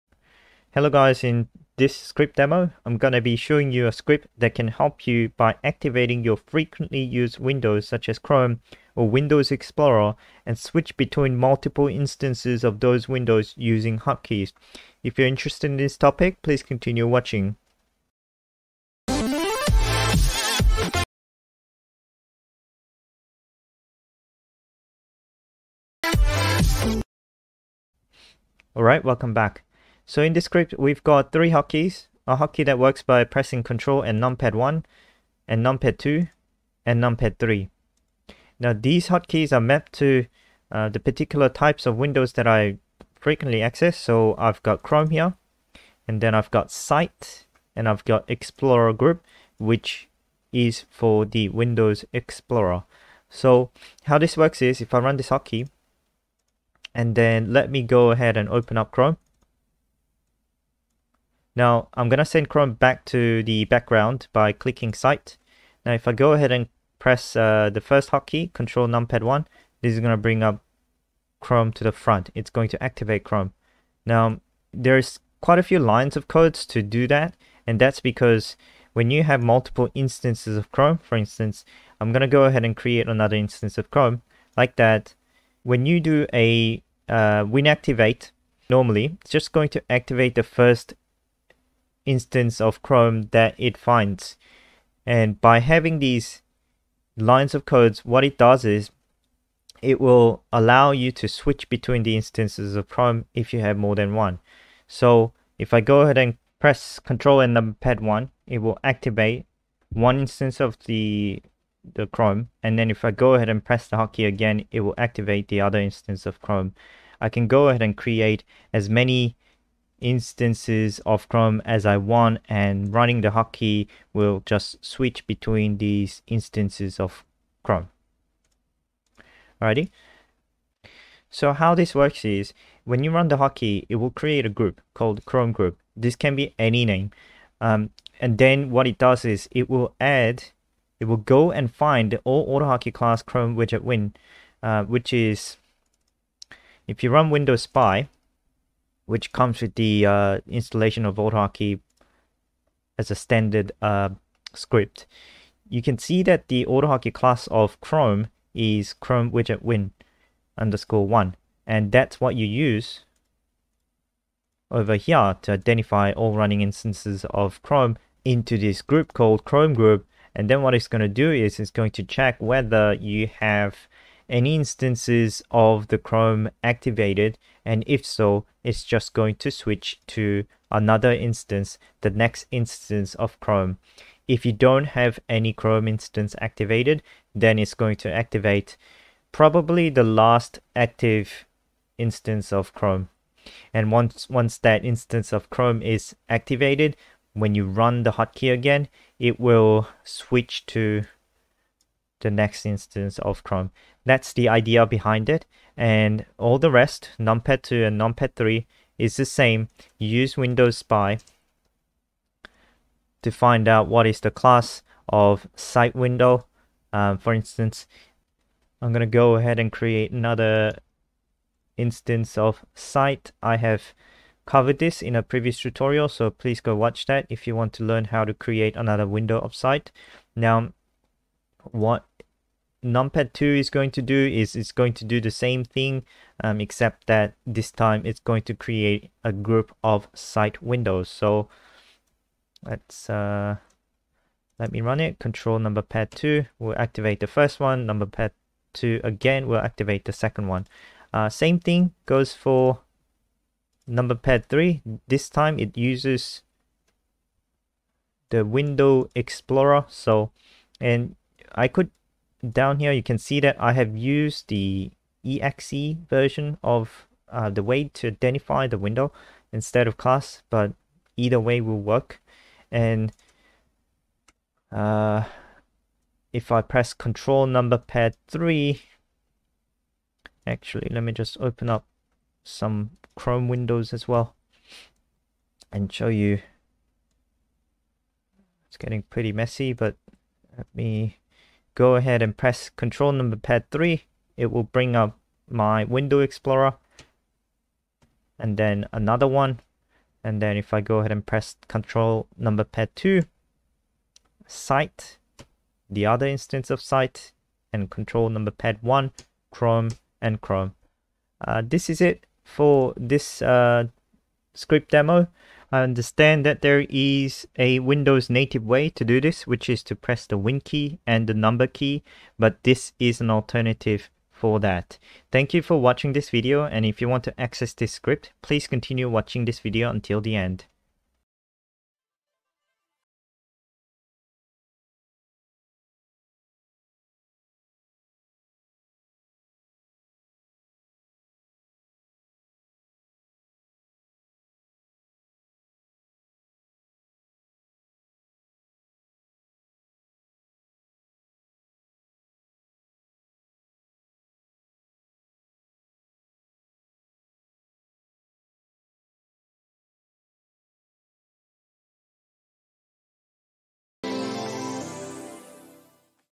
[Juho's AutoHotkey Script Demo #1] Hotkeys To Activate And Switch Between Instances Of Windows
WinActivate command alone will not be able to switch through windows of the same type. In this video, I'll demonstrate to you Group window commands such as GroupAdd and GroupActivate to enable switching through the same type windows.

To take a look at my AutoHotkey tutorial plan:
• select the dropdown list and click the item 'See Tutorial Plan Here'